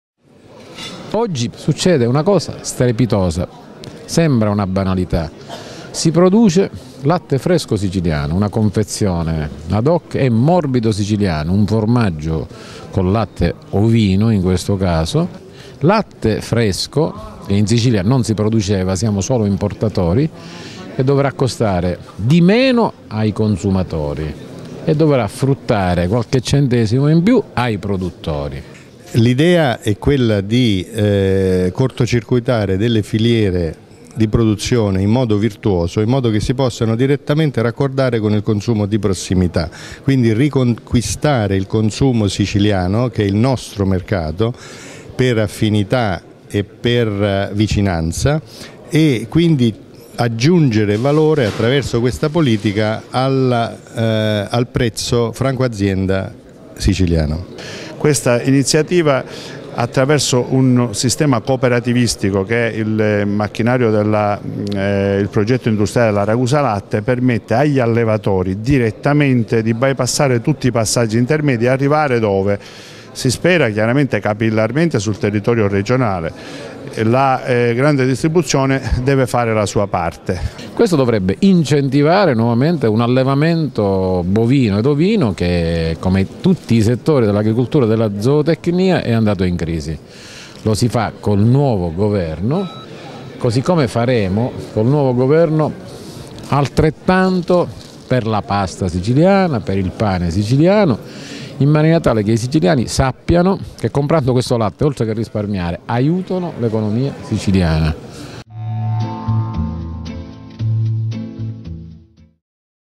Arriva sul mercato il latte fresco "siciliano al 100%"
"L'appuntamento è all'Albergo delle Povere, a Palermo. Il parterre è quello delle grandi occasioni. sale gremita e presenze illustri, a cominicare dal presidente della Regione siciliana, Raffaele Lombardo e dall'assessore regionale alle Risorse agricole e alimentari, Elio D'Antrassi".

Intervista di Antonella Folgheretti. Editing di Davide Vallone.